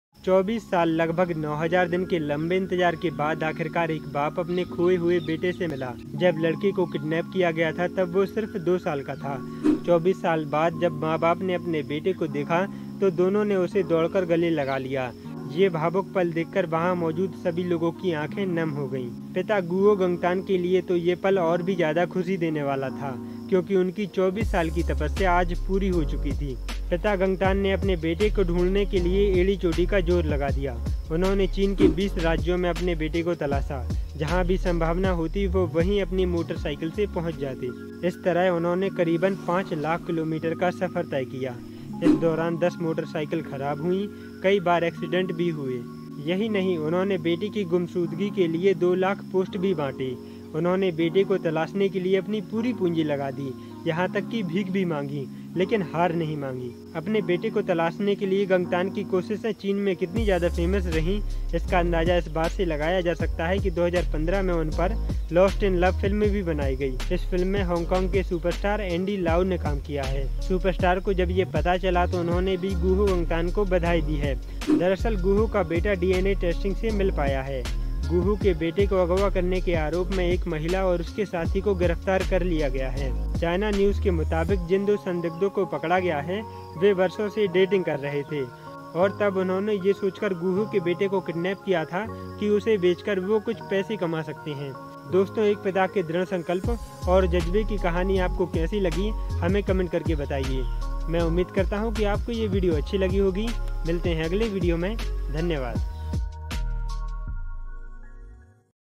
Guo Gangtang ने कैसे खोजा 24 साल पहले चोरी हुआ बेटा
Doston China mein ek Pita ko 24 sal khojne ke bad aakhirkar apna khoya hua beta Mil Hi Gaya. baap bete ke Milan ko dekhte hi mauke per maujud har shaks ki aankhen Nam ho gayi.
         apne bete ko talashne ke liye gangtang ki koshishse China mein itni famous Rahi iska andaza aap is baat se laga sakta hai ki unpar sal 2015 mein Lost in Love film bhi banai gayi.